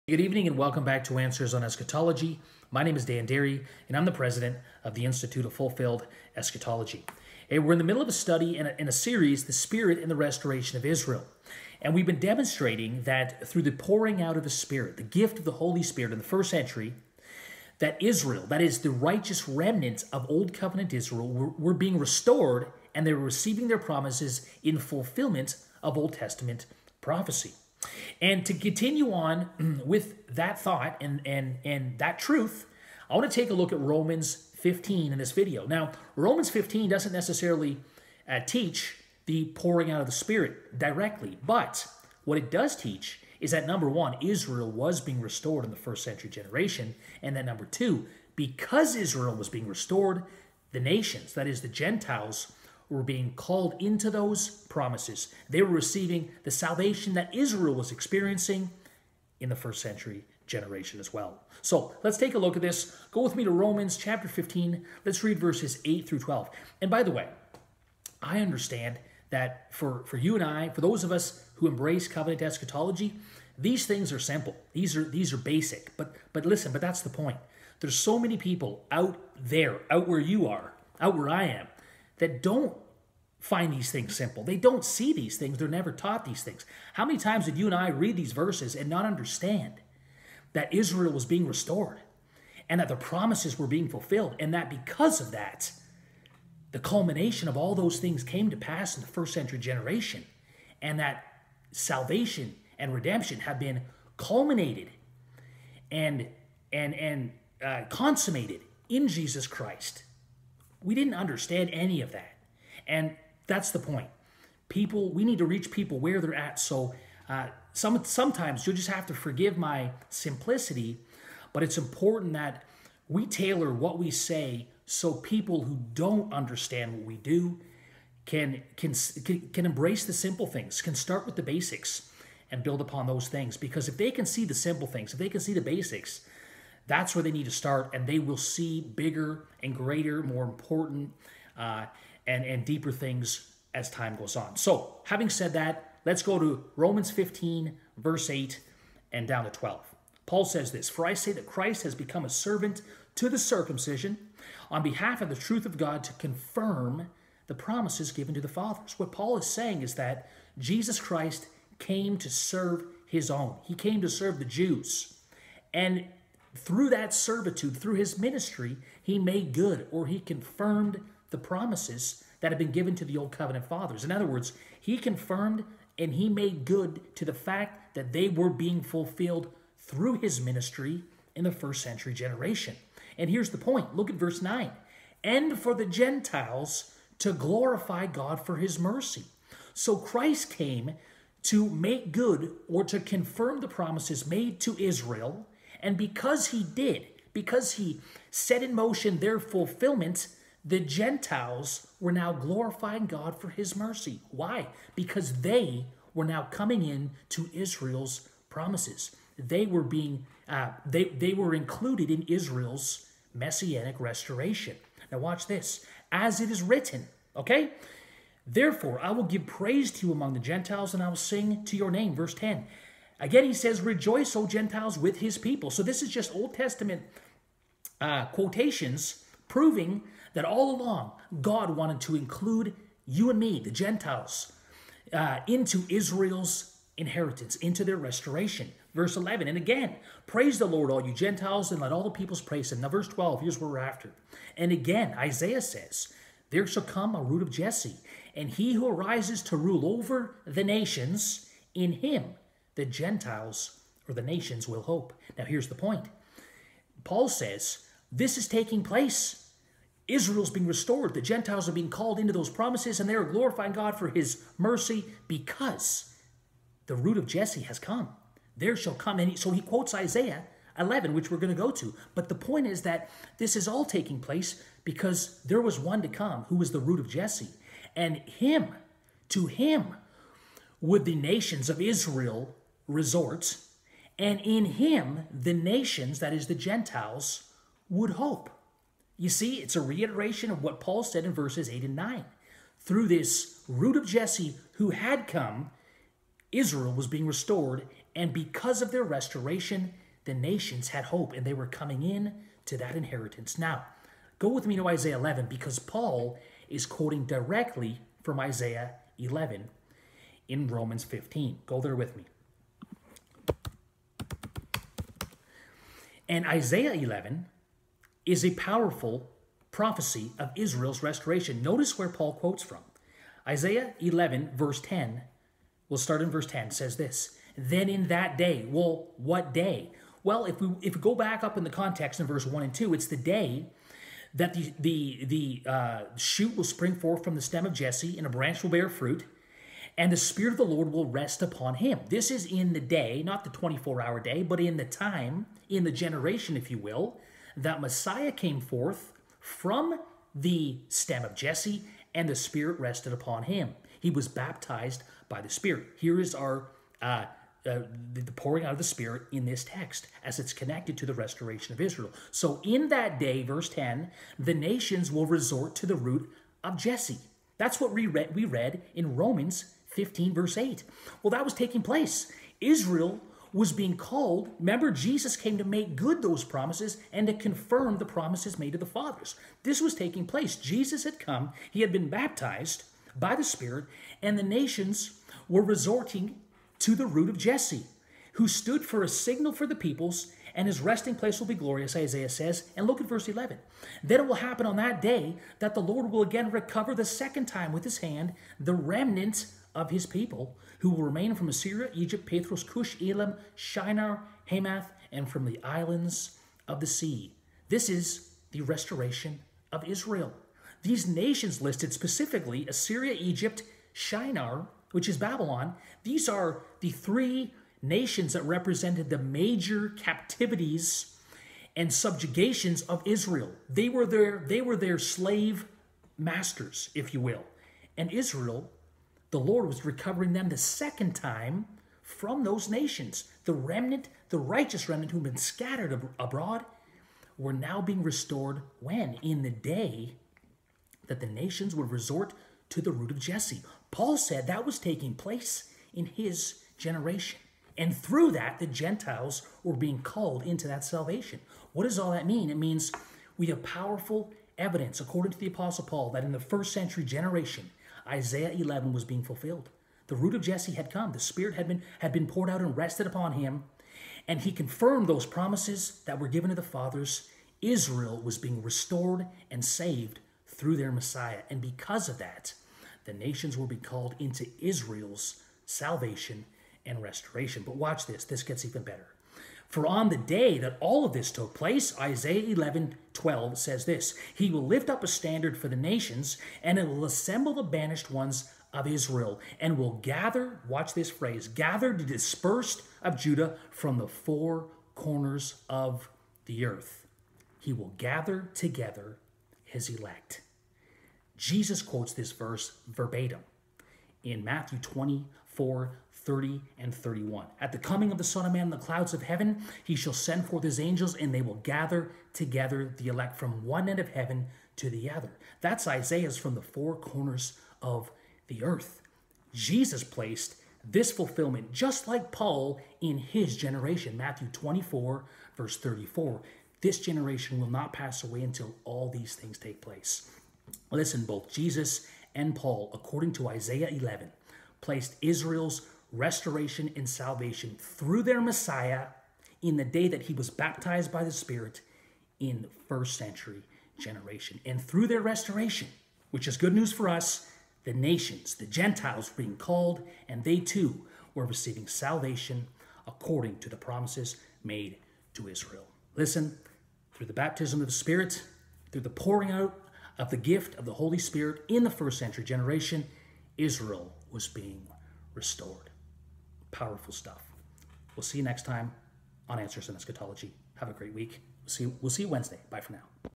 The Spirit & the Restoration of Israel #8 - Glorifying God for His Mercy
According to Paul in Romans 12, it was only through the first century restoration of Israel that the nations (Gentiles) had hope. Paul's point was that since Jesus came and "made good" (confirmed) the promises made to the fathers, those promises (Isaiah 11) were being fulfilled, and the Gentiles were receiving of them also. Only through complete restoration of the remnant of Israel can the nations now "glorify God for his mercy".